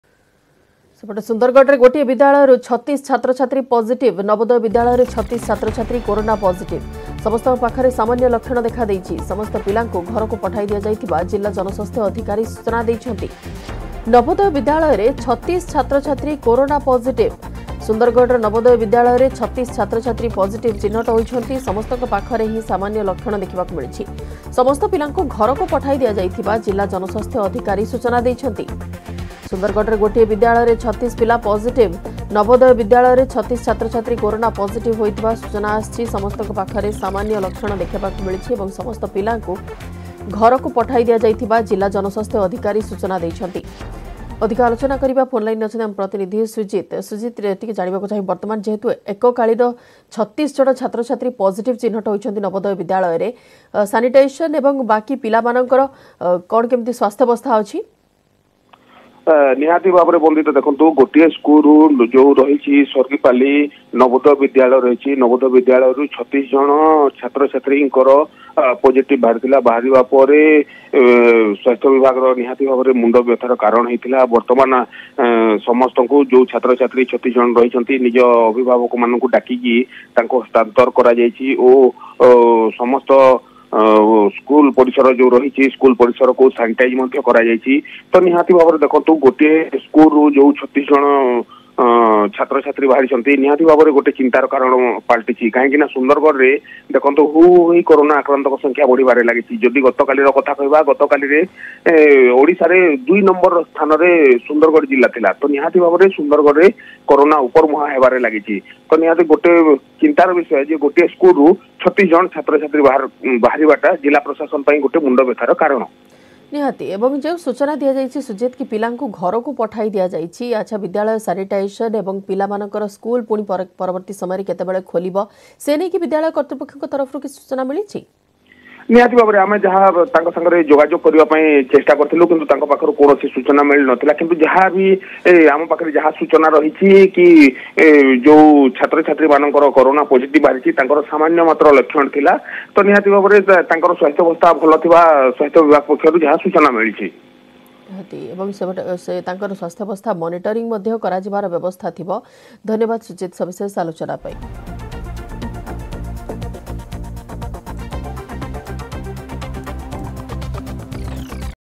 Sundargarh Corona Case: 36 New Positive Cases Found In Sundargarh Navodaya School
Sundargarh Corona Case: 36 New Positive Cases Found In Sundargarh Navodaya School I There is minor symptoms found in students I 5th Jan 2022